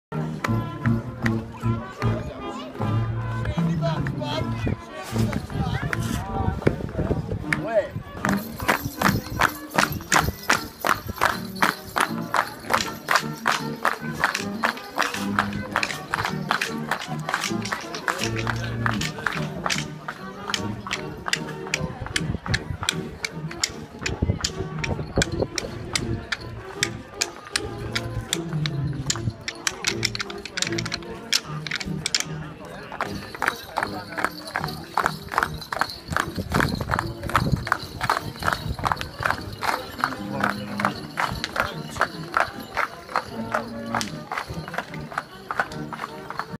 Britannia Coconutters
Easter Saturday 2014
Bacup
Boundary to Boundary dance